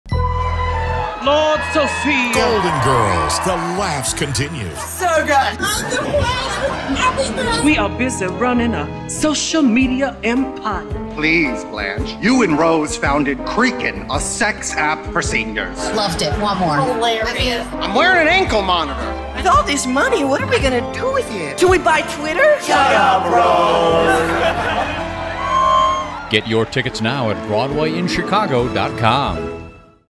Golden Girls: The Laughs Continue | Broadway In Chicago
Golden Girls: The Laughs Continue
Thank You for Being a Parody

Playing at: Broadway Playhouse
Dates: February 6 - February 25, 2024
Runtime: 1 hour and 40 minutes with one 20 minute intermission

Picture it. United States, 2024. GOLDEN GIRLS: THE LAUGHS CONTINUE is a parody that brings Miami’s sassiest seniors to stages around the country for one more hurrah. We find Sophia out on bail after being busted by the DEA for running a drug ring for retirees. Blanche and Rose have founded CreakN, a thriving sex app for seniors. And Dorothy is trying to hold it all together with help from a new (much) younger sex-crazed lover. GOLDEN GIRLS: THE LAUGHS CONTINUE allows audiences to relive the heartfelt hilarity of the four ladies who never stopped being best friends.

GOLDEN GIRLS: THE LAUGHS CONTINUE is a professional stage show unlike any other Golden Girls tribute that you might have experienced. The play is written by Robert Leleux, author of The Memoirs of a Beautiful Boy and The Living End. His work has also appeared in The New York Times and The New York Times Magazine and his other plays have been produced across the country. The show is directed by Eric Swanson, who was the co-founder and Executive Director of The Detroit Actors’ Theatre Company. The cast includes Ryan Bernier as Dorothy, Vince Kelley as Blanche, Adam Graber as Rose, Christopher Kamm as Sophia, and Jason Bowen as Stanley/Burt. The tour is produced by Murray & Peter Present and Innovation Arts & Entertainment.

Adult content. Recommended for Ages 18+